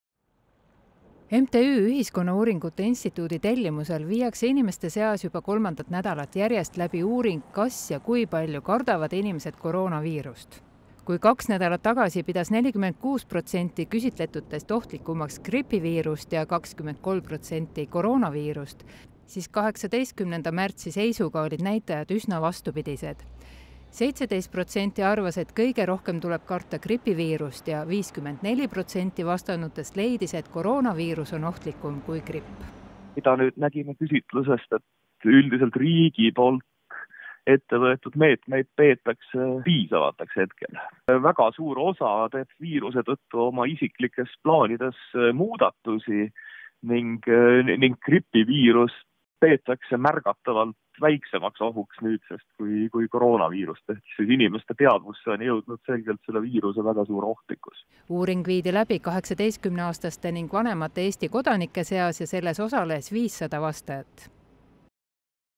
19.03.2020 - Uuring: Koroonaviiruse ohu tajumine on märgatavalt kasvanud
Koroonaviirus on jõudnud inimeste teadvusesse ja seda kardetakse juba üle kolme korra enam kui gripiviirust, selgus sel nädalal läbi viidud uuringust.